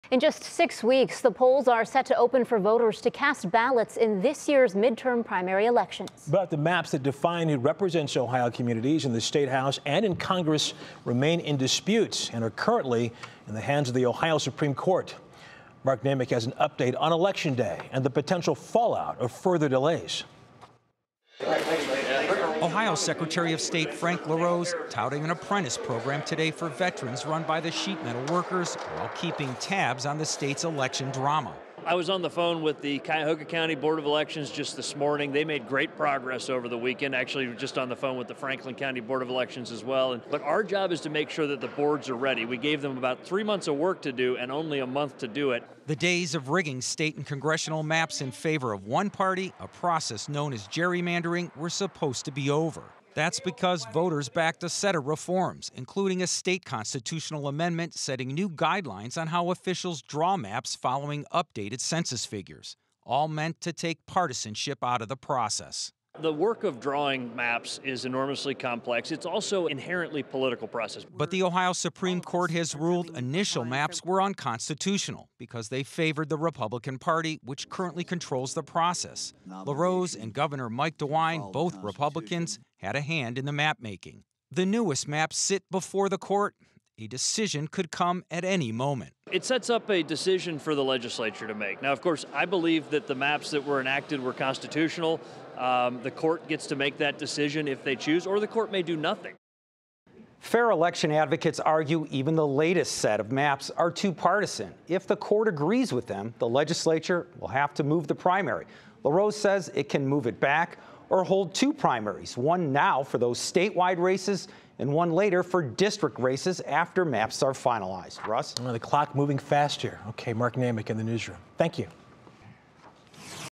Political theater in Ohio threatens May primary
In May, the polls in Ohio are set to open for voters to cast ballots in this year’s midterm primary elections.

But the maps that define the boundaries – and ultimately who will represent Ohio communities in Columbus and in Congress -- remain in dispute and are currently in the hands of the Ohio Supreme Court.

3News asked Ohio Secretary of State Frank LaRose Monday in Parma for the latest on the state’s election drama. He was touring an apprentice program for veterans run by the Sheet Metal Workers.

“I was just on the phone with Cuyahoga County Board of Elections and they made great progress over the weekend, and I was just on with Franklin County Board of elections as well,” he said about the rushed preparations caused by mapmaking delays. “Our job is to make sure the boards of elections are ready. We gave them about three months of work to do and only about one month to do it.”

The days of rigging state and congressional maps to favor one party – a process of known as gerrymandering -- were supposed to be over. That’s because voters backed reforms, including a 2015 constitutional amendment setting new guidelines on how officials draw maps following updated U.S. Census figures. The reforms are intended to remove partisanship from the process.

“The work of drawing maps is enormously complex, and it is an inherently political process,” said LaRose.

But the Ohio Supreme Courts has ruled initial maps were unconstitutional because they favored the Republican Party, which currently controls the process. LaRose and Gov. Mike DeWine, both Republicans, had a hand in the mapmaking.

The newest maps sit before the court. A decision that could come any moment.

“This sets up a decision for the legislature to make,” he said. “Of course, I believe the maps enacted were constitutional. The court gets to make that decision or the court could do nothing.”
If the court the does nothing, the latest maps stand. If the Court rejects the latest maps, elections officials will have a hard time preparing for the May primary. The legislature has the power to move the primary. LaRose says it can move the entire election back or hold two primaries – one for statewide races now and one later after legal challenges to the maps are resolved.